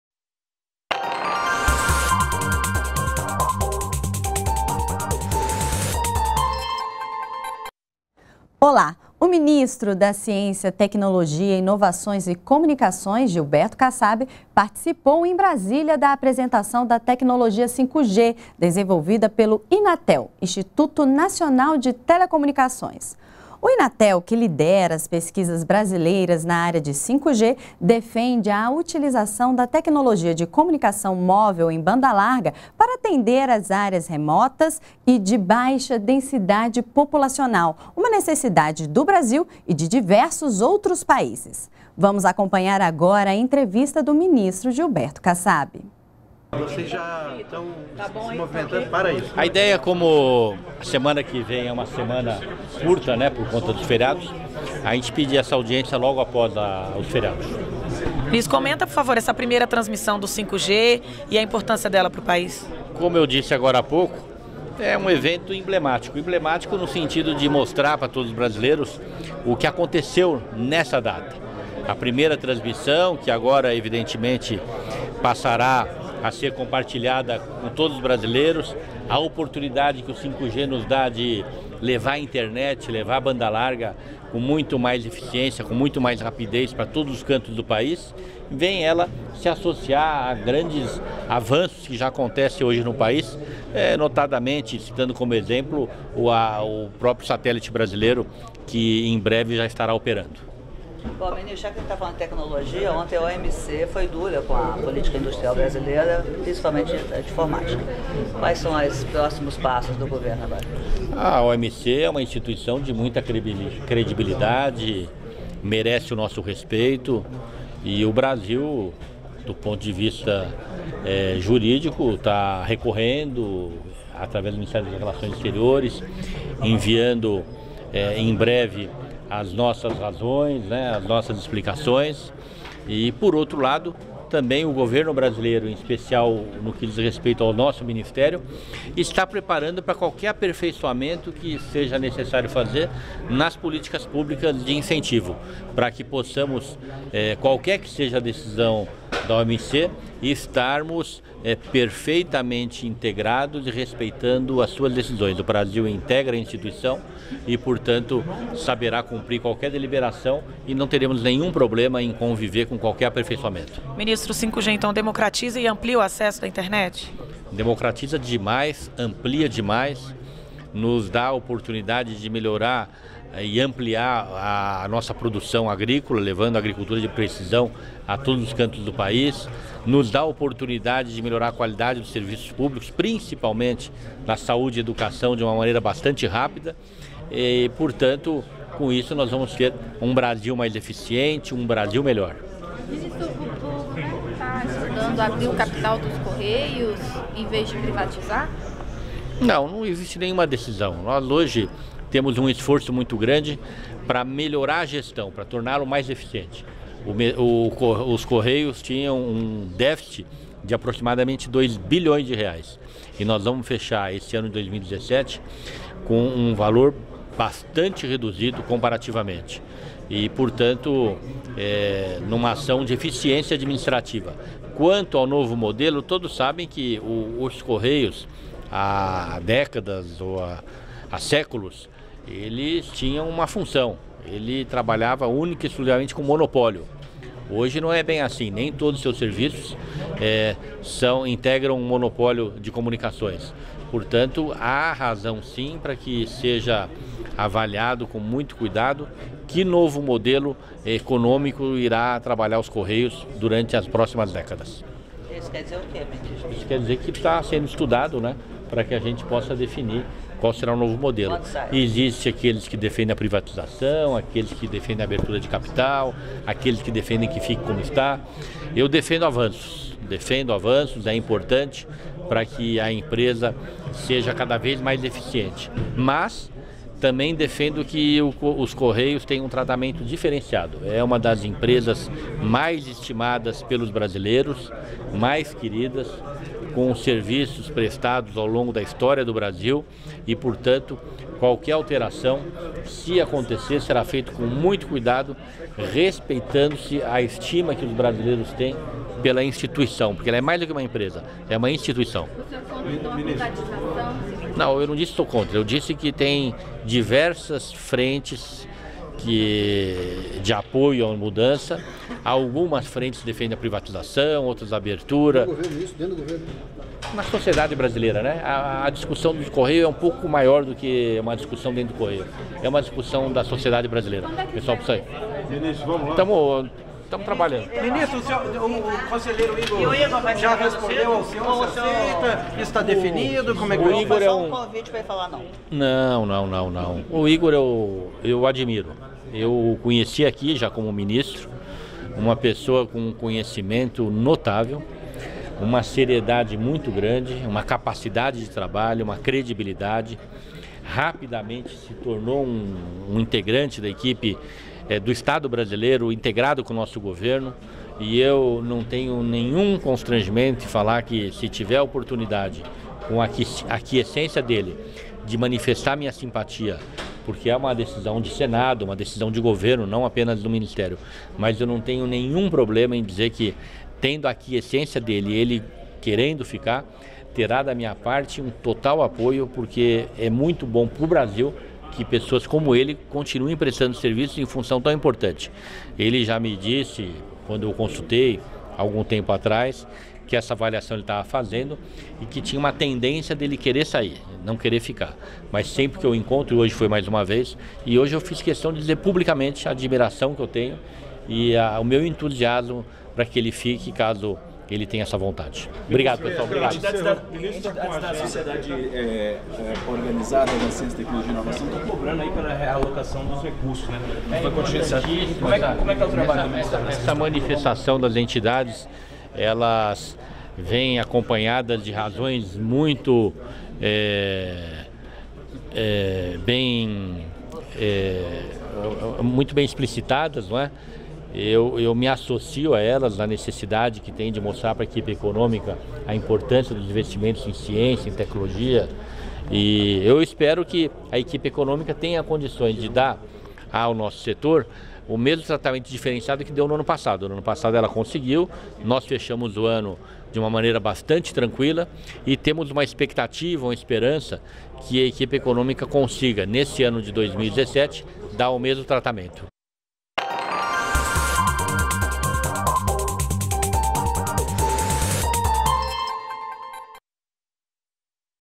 Ministro Gilberto Kassab comenta a tecnologia 5G
FIQUE POR DENTRO - 31.08.17: Ministro da Ciência, Tecnologia, Inovações e Comunicações, Gilberto Kassab, conversa com jornalistas após apresentação da tecnologia 5G.